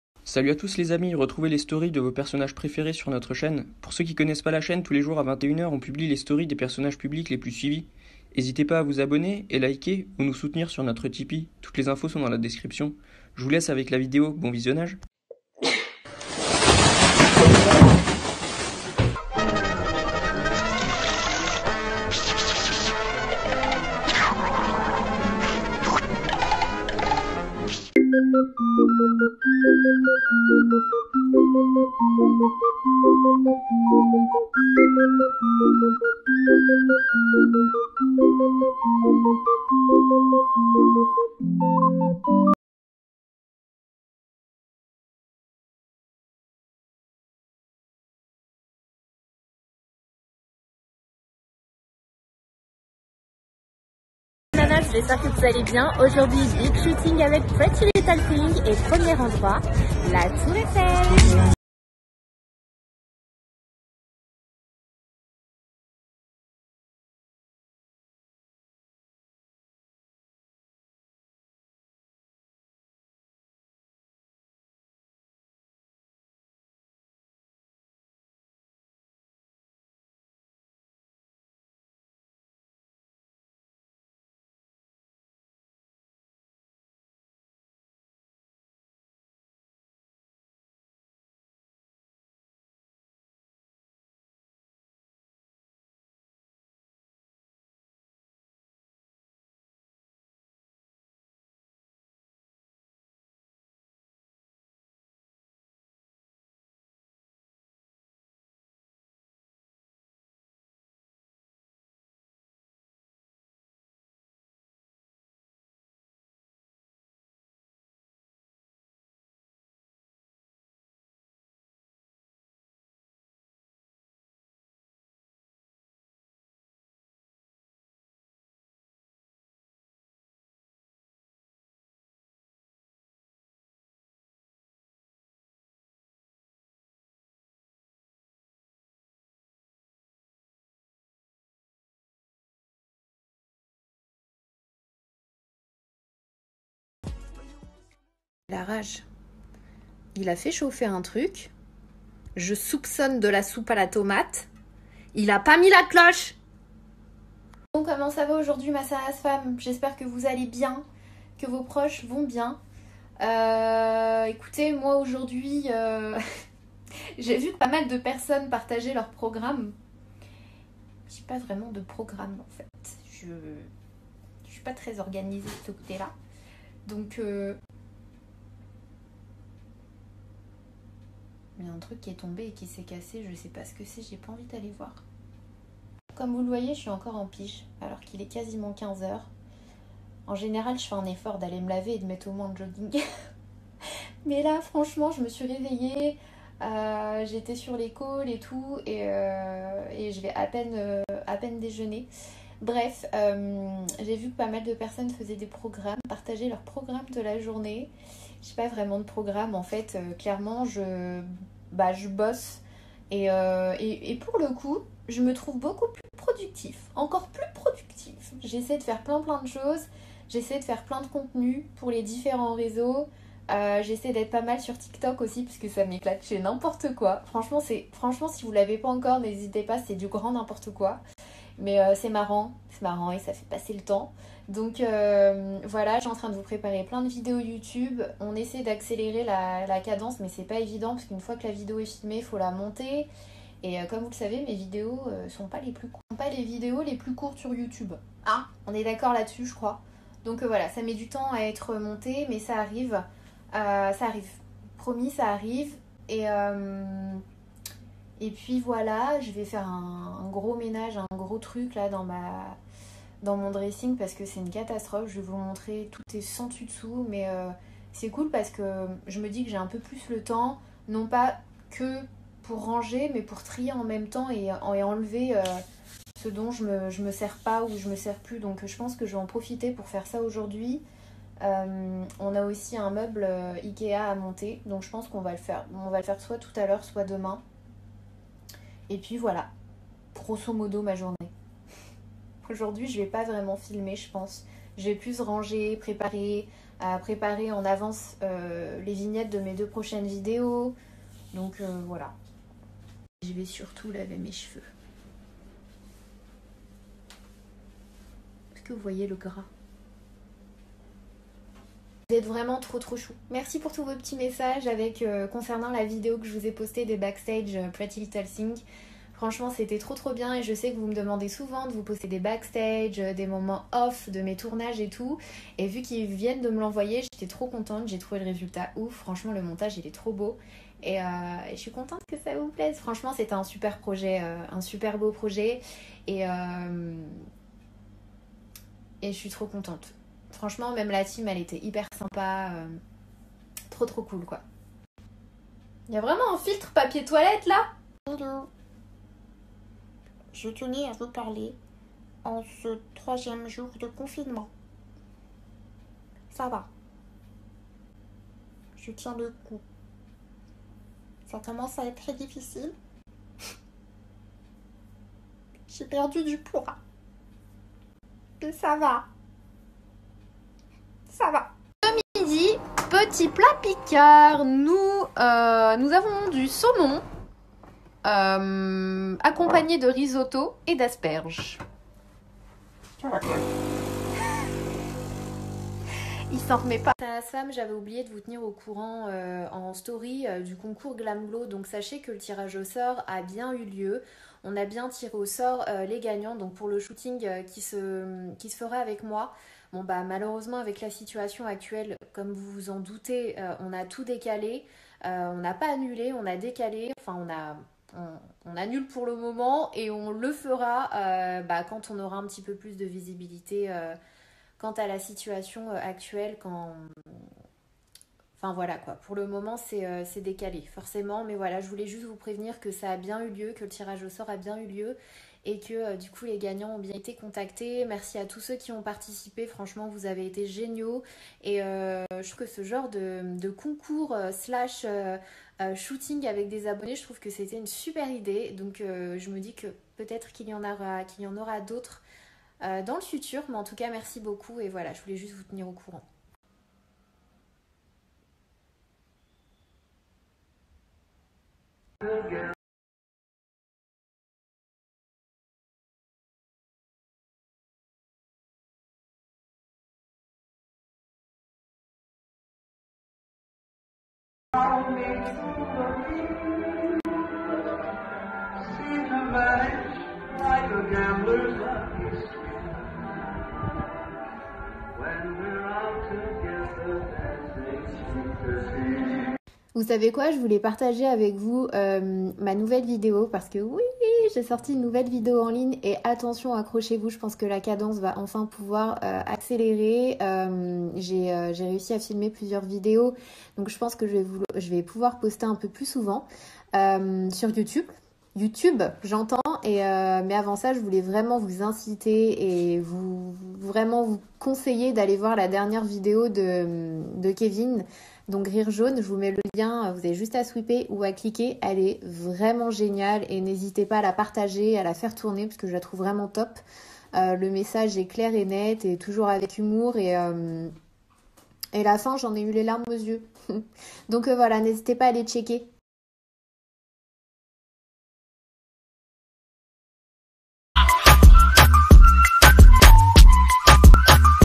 SANANAS 24H STORY INSTAGRAM - 20 Mars 2020
Revivez en intégralité les Story de Sananas le 20 Mars 2020 - Pour plus de story consultez notre playlist Sananas

Bon visionnage :)